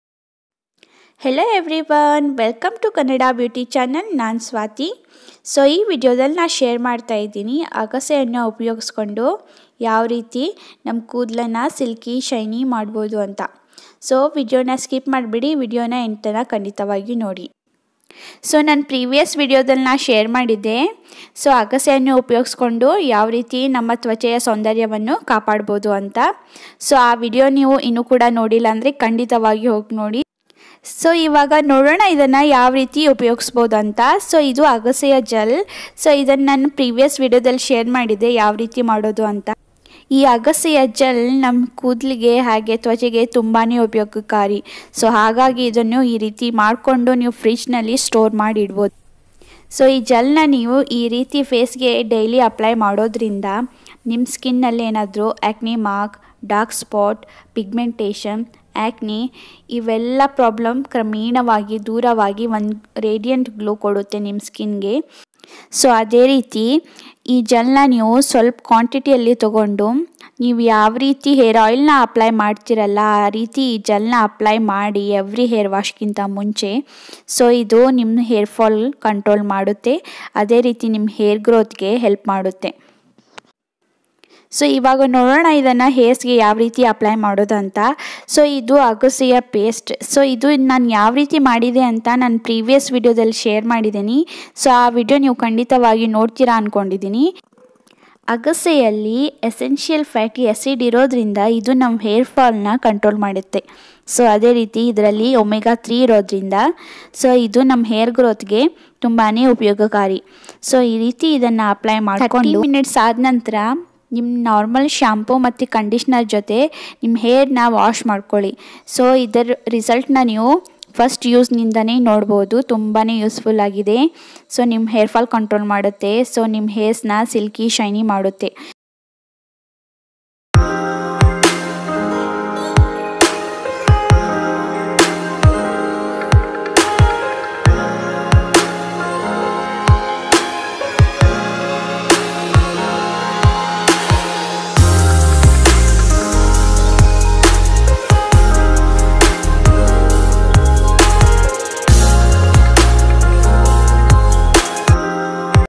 ವಾರಕ್ಕೆ ಒಮ್ಮೆ ಇದನ್ನು ಹಚ್ಚಿ | Flax Seed Gel For Triple Hair Growth
Hi
I am swati,

I am going to share this ಕೂದಲಿಗೆ ಆರೈಕೆಗೆ agase hair pack,flax seed hair pack for silky shiny hairs,beauty tips,kannada beauty channel your feedback